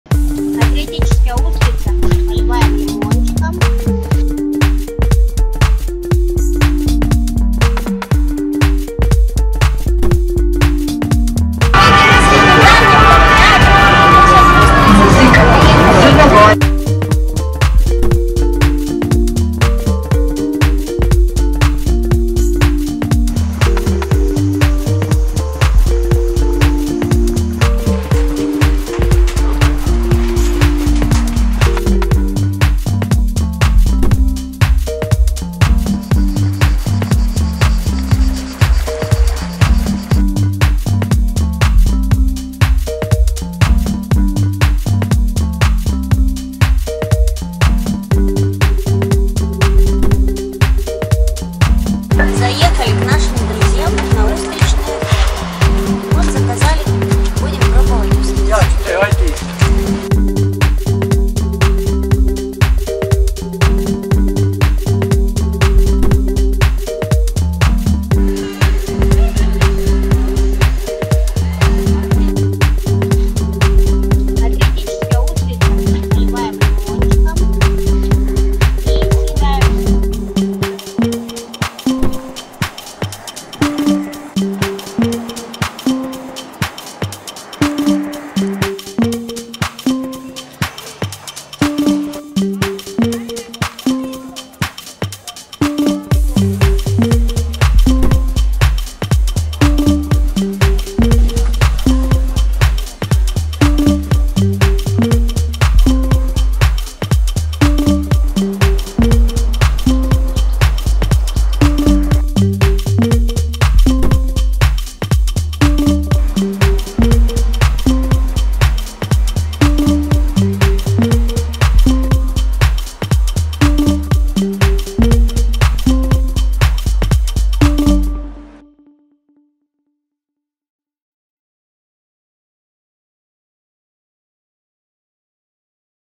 Oysters at an oyster farm, Montenegro.
From Herceg Novi we went to the ferry to Lipetani. On the ferry from Lipetani we got to Kamenari. We visited our friends at the oyster farm. Then we went back on the ferry, when we were returning we saw warships.

Это тоже интересное для Вас: Питание и цены в Черногории Пляжи в Черногории Апартаменты в Черногории Самостоятельные путушествия по Черногории